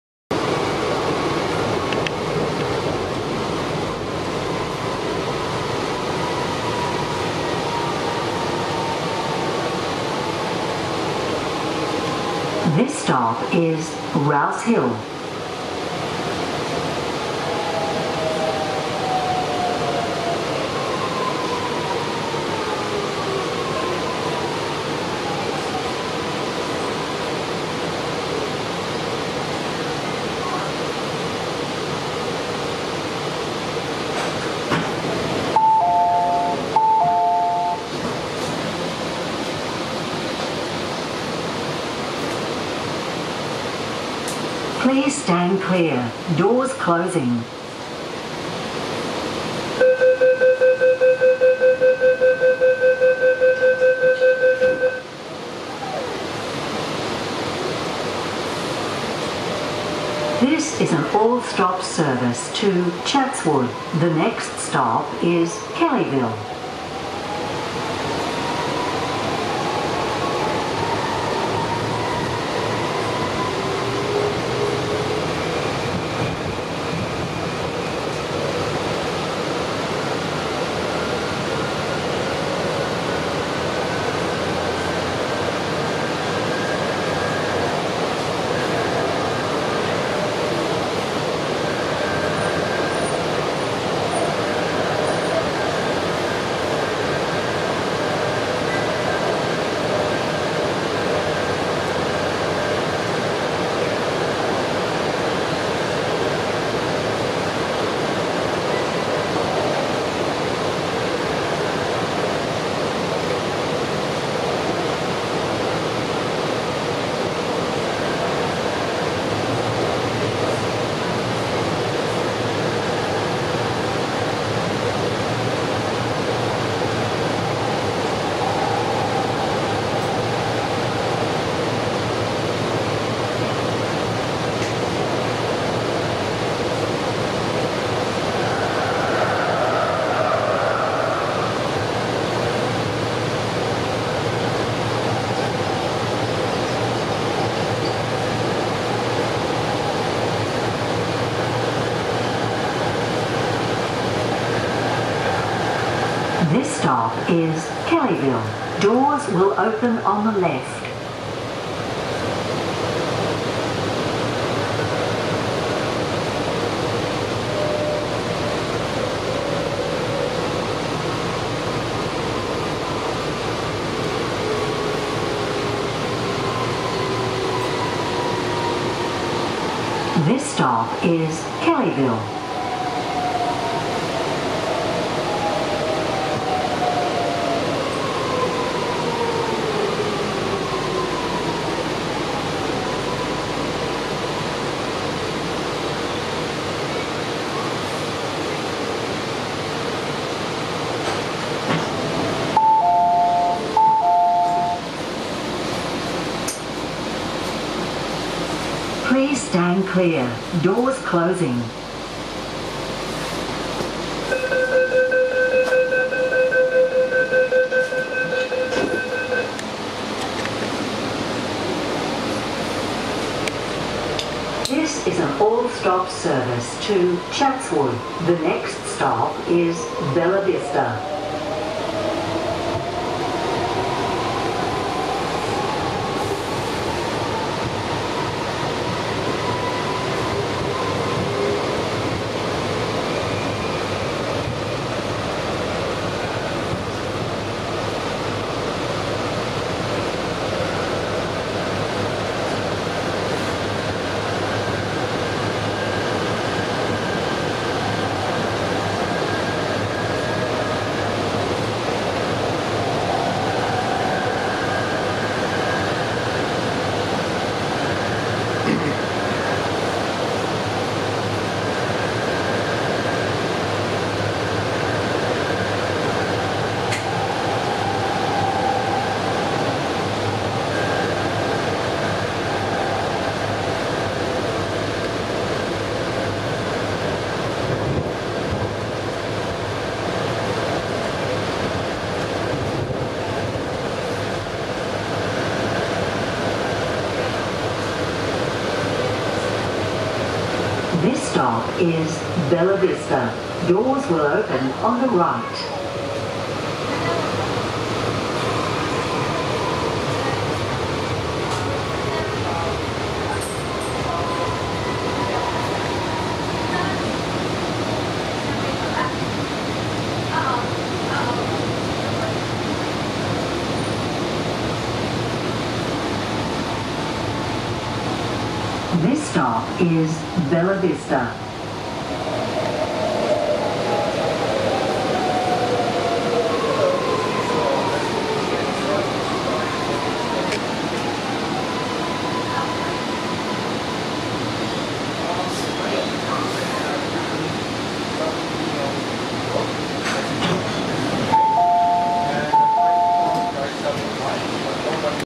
Sydney Metro Metropolis (Batch 1) [02]: Rouse Hill → Bella Vista
20 March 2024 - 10:52 to 10:58 am. Sydney Metro announcements and Dynamic Route Map Displays have been standardised on all 45 trains. This is the first time hearing Taylor Owynns over the PA on Sydney Metro.

Quick shoutout to: @PHope-sl4nj